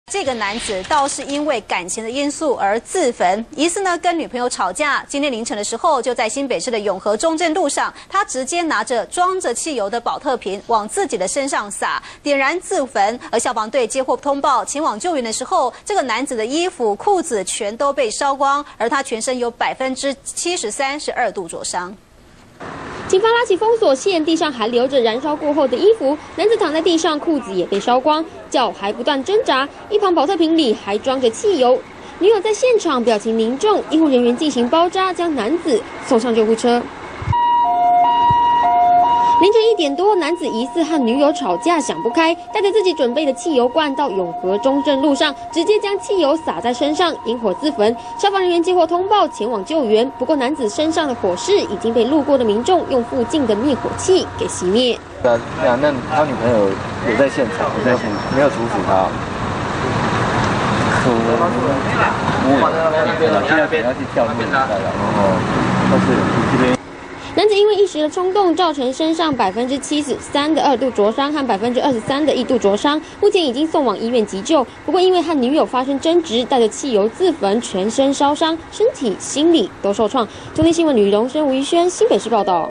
感情糾紛？男自焚 全身73%二度灼傷
中天新聞》新北市一名男子疑似跟女友發生爭吵，情緒激動引火自焚，消防局接獲報案，到現場救援，男子全身遭到火舌吞噬，燒傷面積有73%二度灼傷，馬上被送醫急救，目前男子還在醫院搶救中。